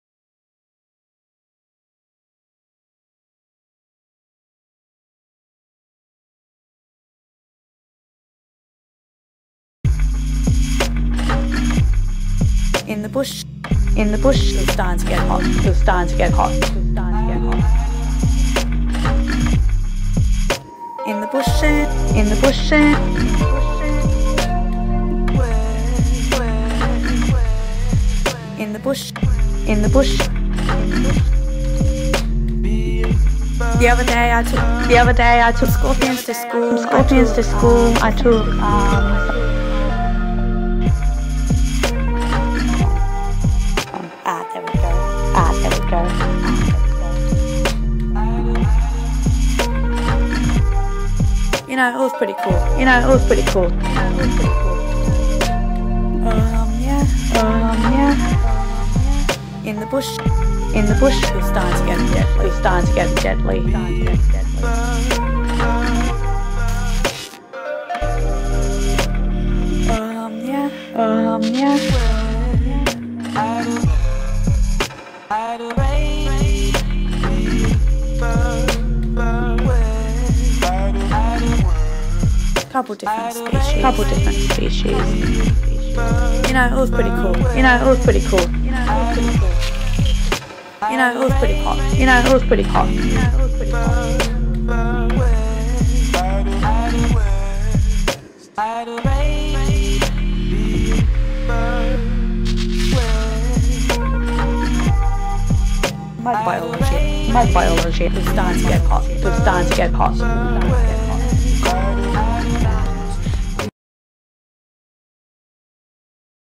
Ambystoma mavortium hypomelanistic larvae
I noticed this odd larvae early on. Here is a video of what it looks like. Plz comment and share thoughts